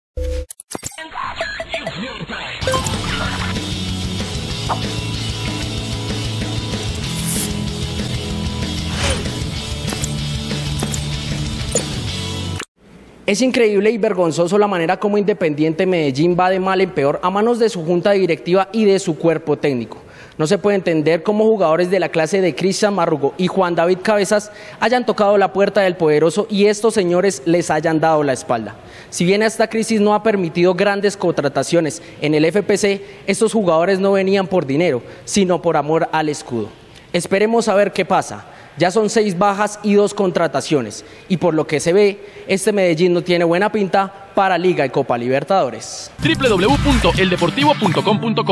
Grave lo del DIM con Cristian Marrugo
No se puede entender cómo el club le cerró la puerta a uno de los referentes del último tiempo.